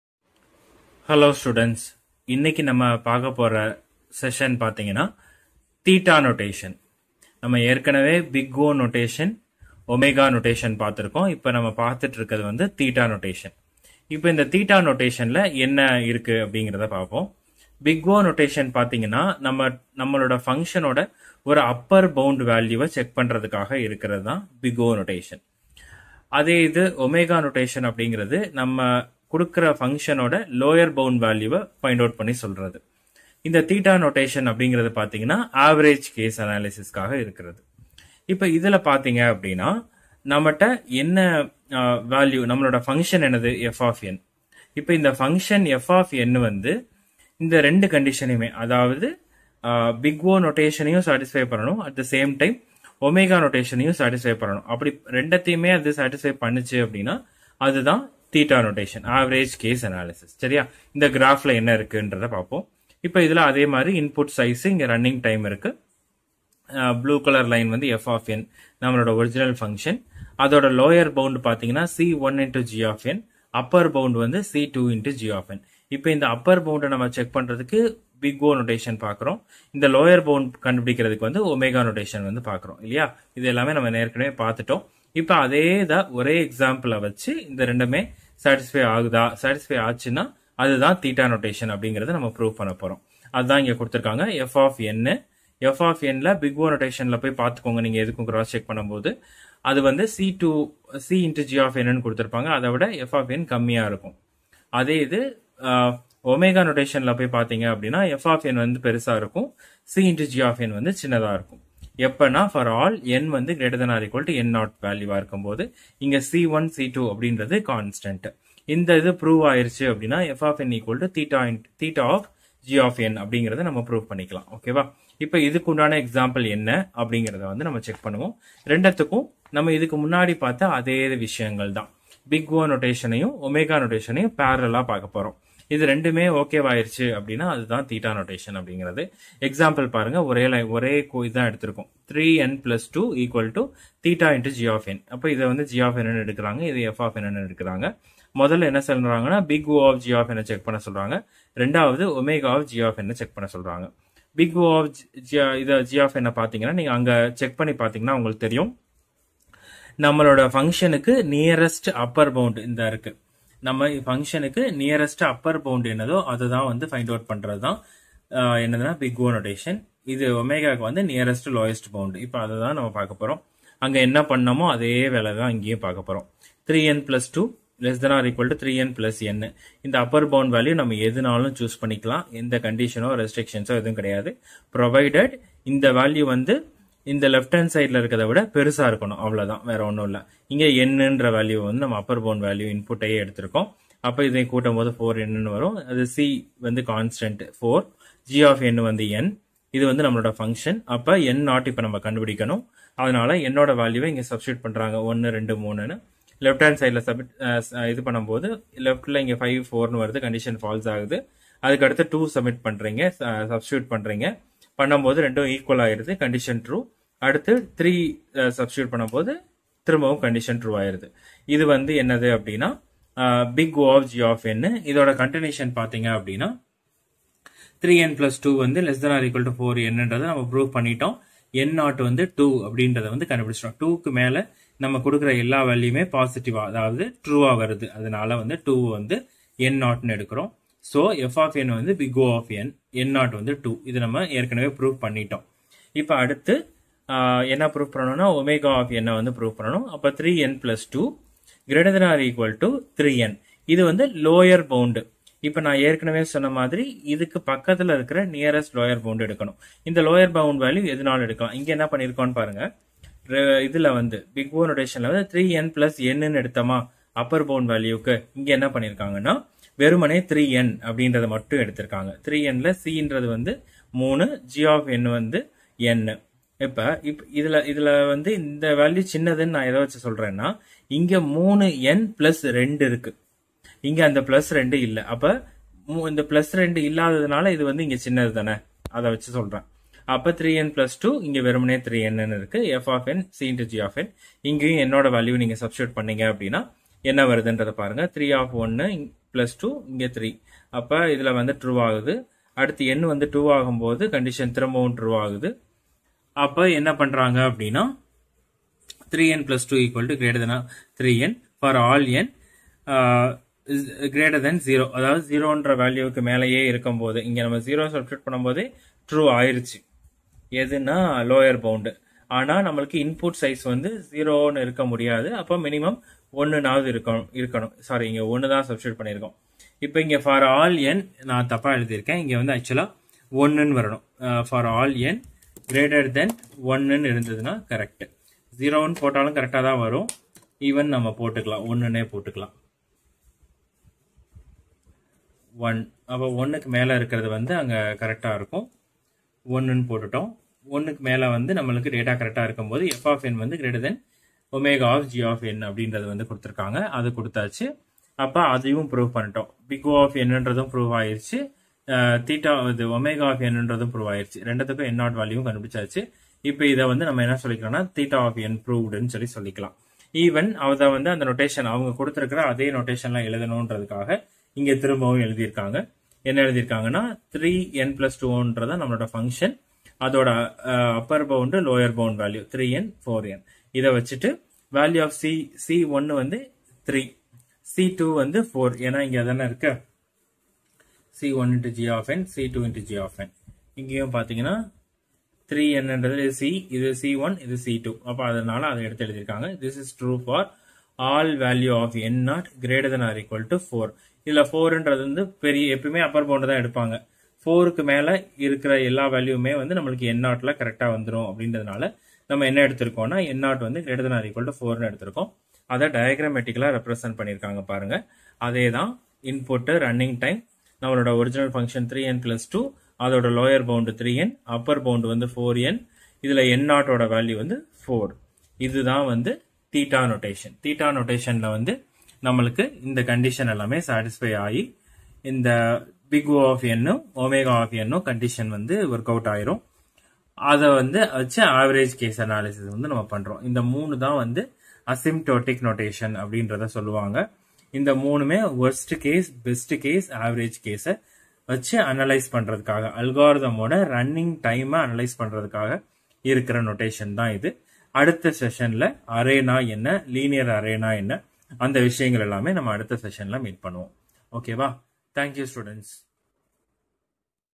DS TUTORIAL- 6 | Data Structure Theta Notation Explain | Dr.M.Sukumar, Ph.D. | Tamil | M42 TECH
DS TUTORIAL- 6 | Data Structure Theta Notation Explain | Dr.M.Sukumar, Ph.D. | Tamil | M42 TECH Wesite